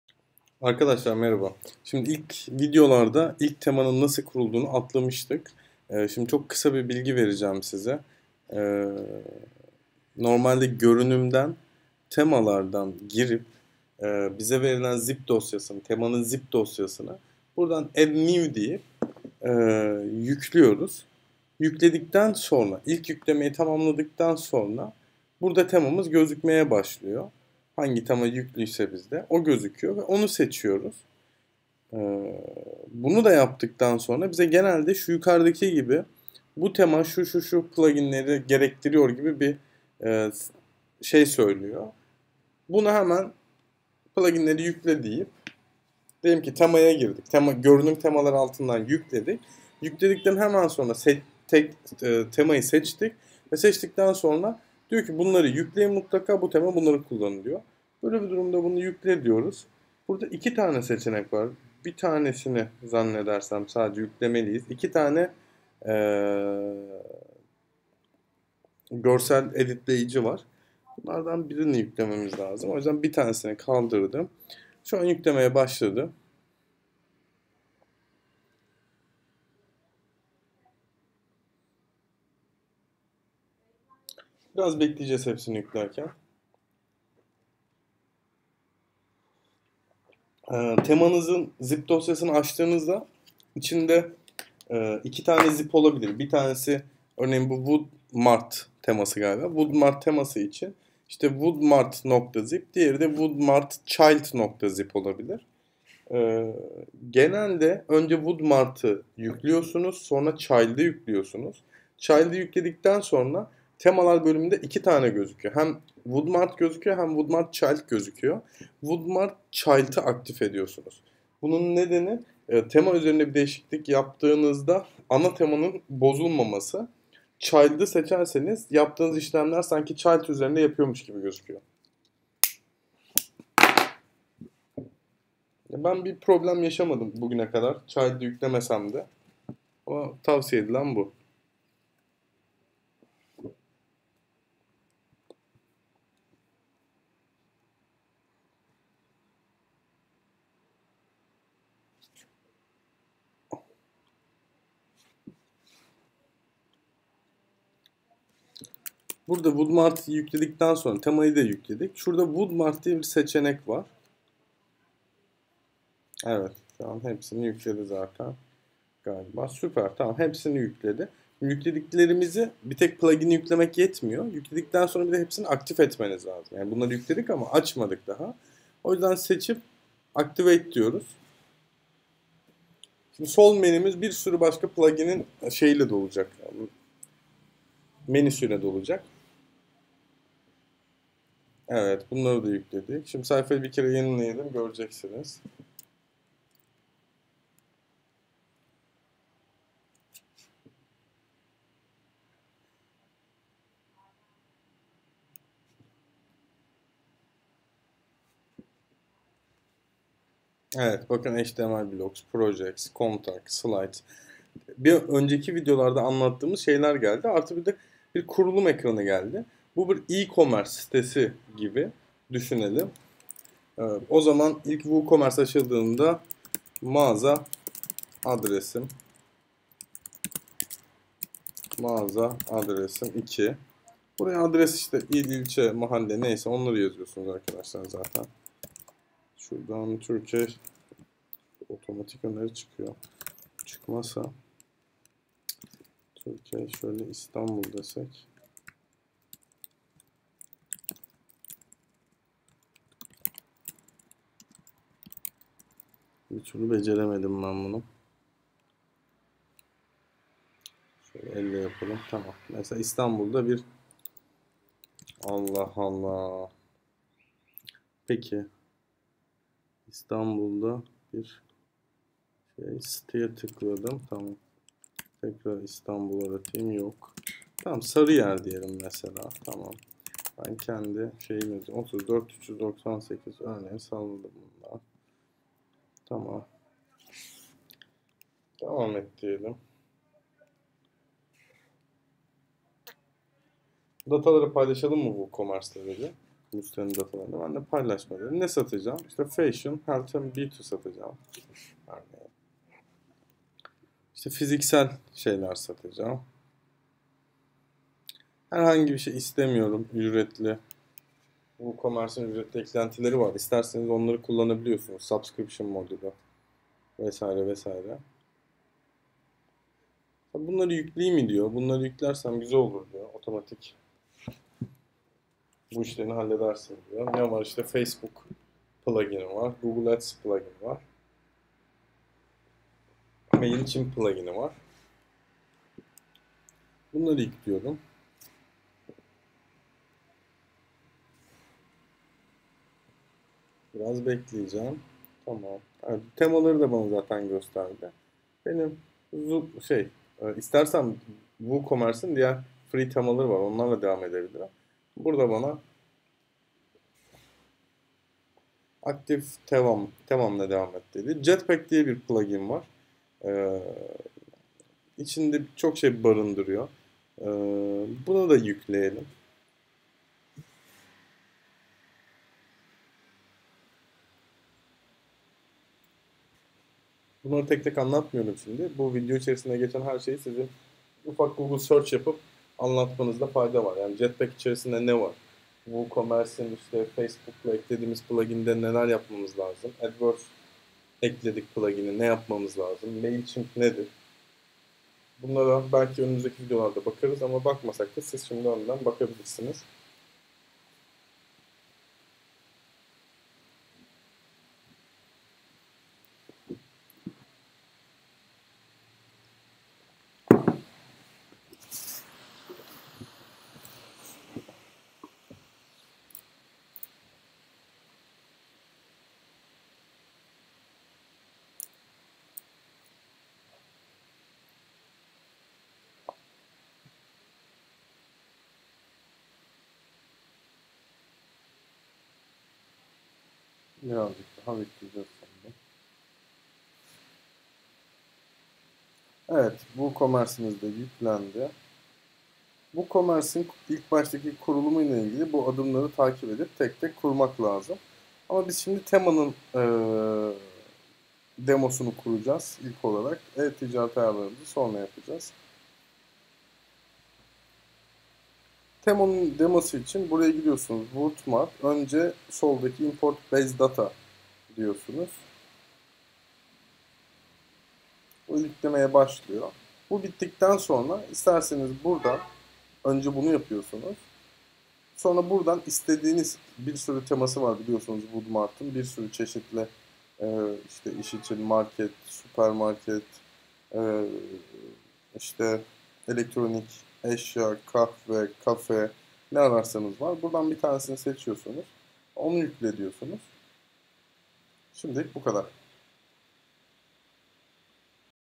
Satın Alınan Temanın Kurulumu ve WooCommerce Eklentisinin Eklenmesi - Ders 3 - Wordpress Workshop
Tema yükleme ve etkinleştirme konusunu tekrar ediyoruz. Bu sefer bir WooCommerce teması kurduktan sonra temanın ihtiyacı olan eklentileri yüklüyoruz.

00:00 Tema Kurulumu
00:36 Tema Eklentilerini Yükleme
03:34 WooCommerce Kurulumu
07:33 Demo Tema İçeriklerini Yükleme

Çağdaş Kurultay Kalkan;